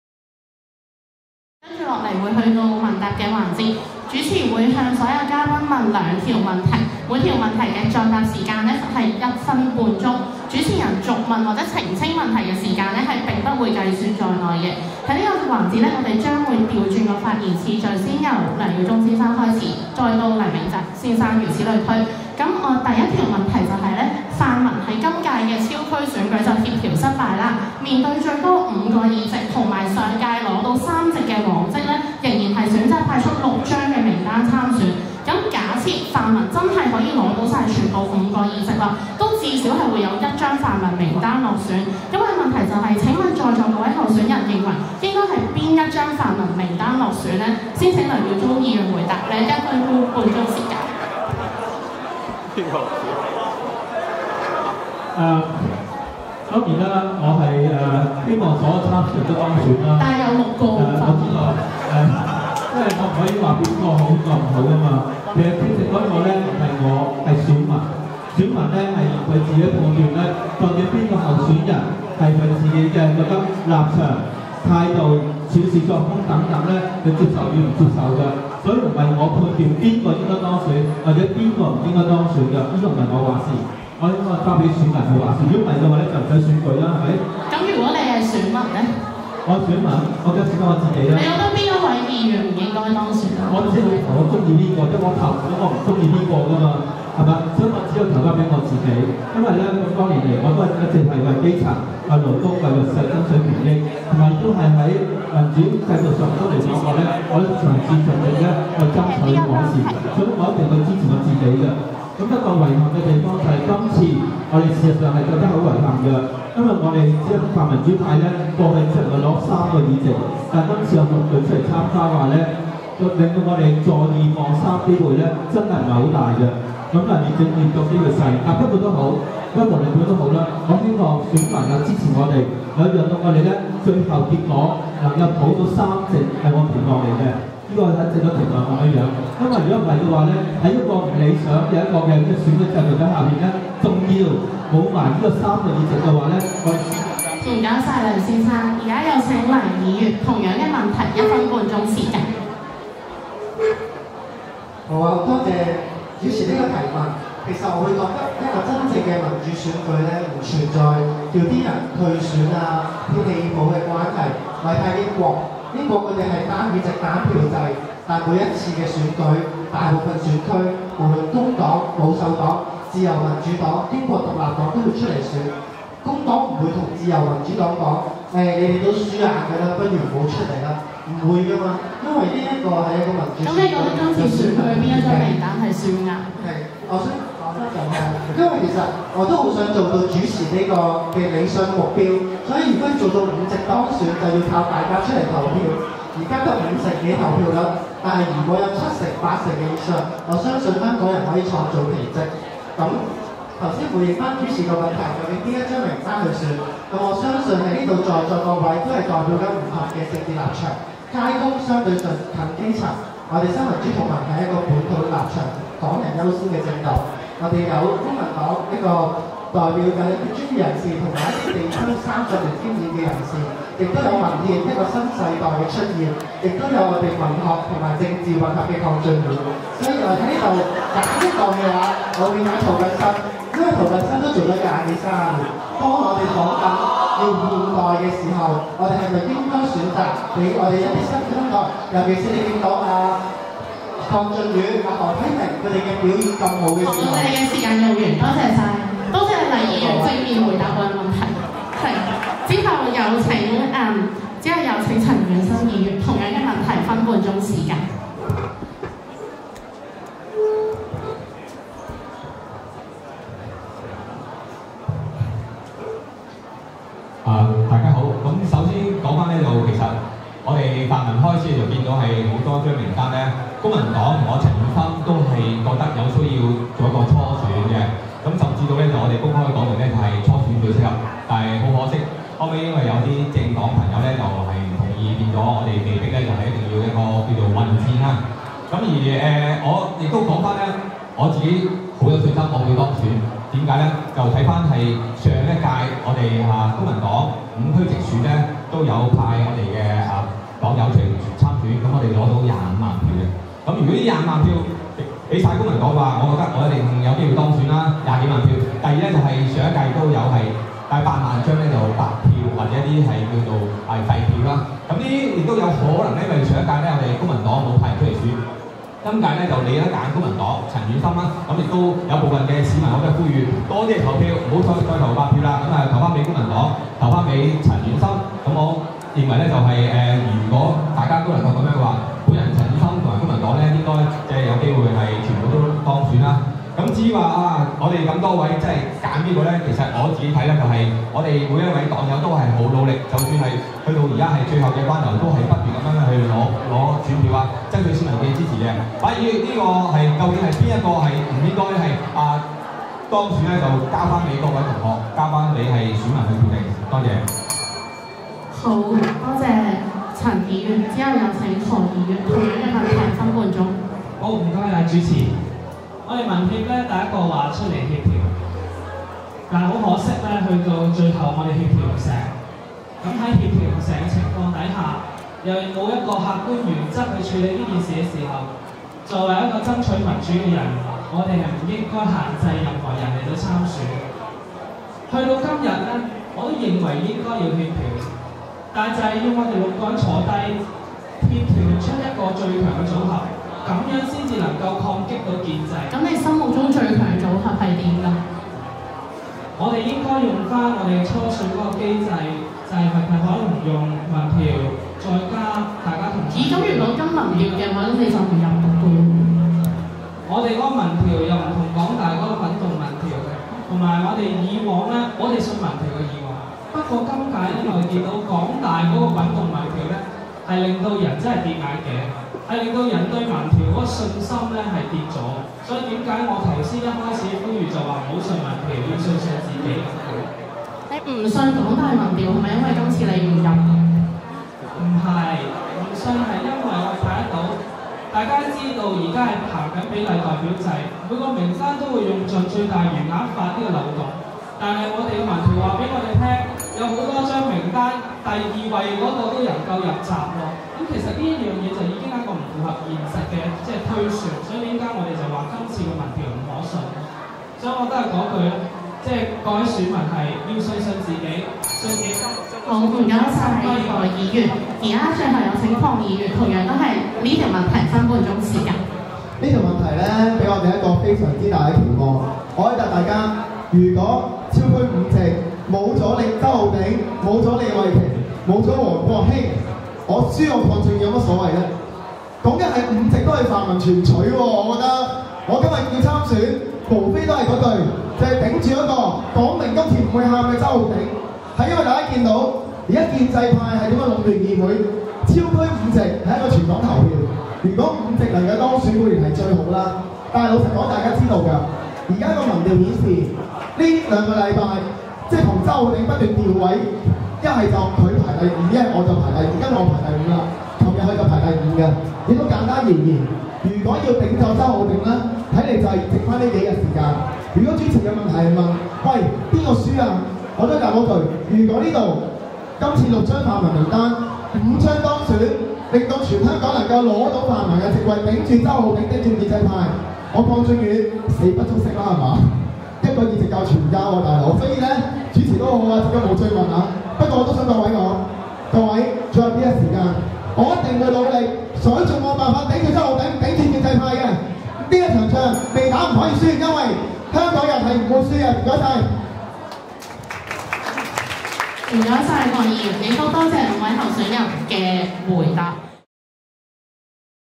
01SEP2016 統一提問第一條：如果超區五席泛民全取，六位泛民候選人你最想誰出局？ -「立法會區議會(第二) 功能界別 (超級區議會) 選舉論壇」- 港大政政 (2/8)
立法會所有參選人資料